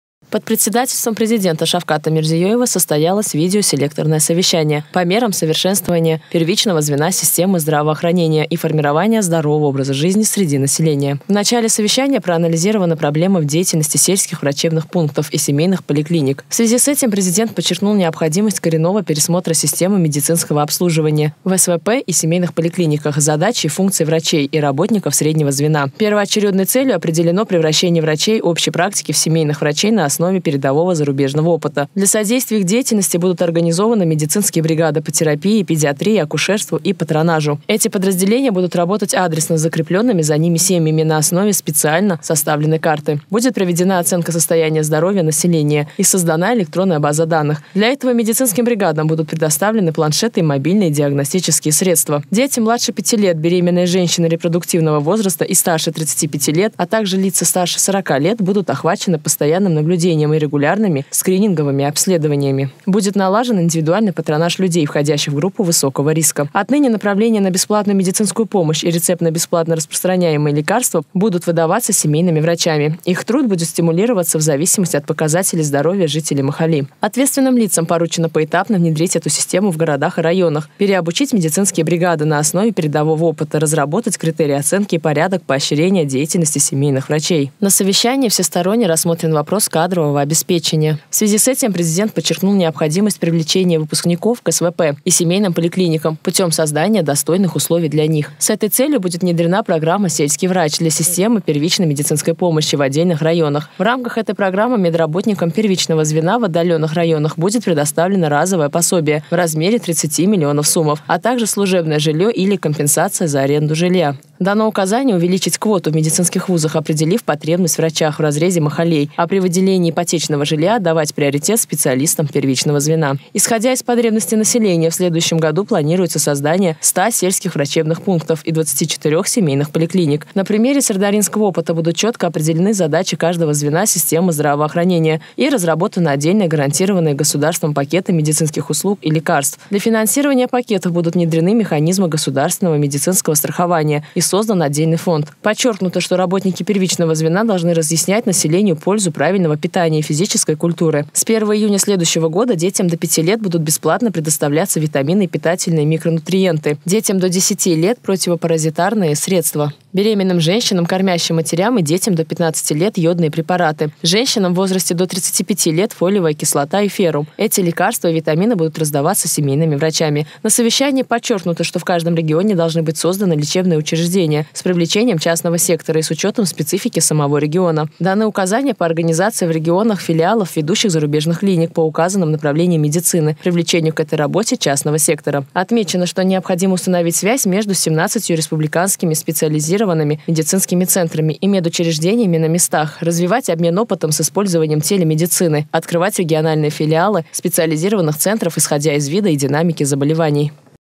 Определены меры по совершенствованию первичного звена здравоохранения
Под председательством Президента Шавката Мирзиёева 6 ноября состоялось видеоселекторное совещание по мерам совершенствования первичного звена системы здравоохранения и формирования здорового образа жизни среди населения.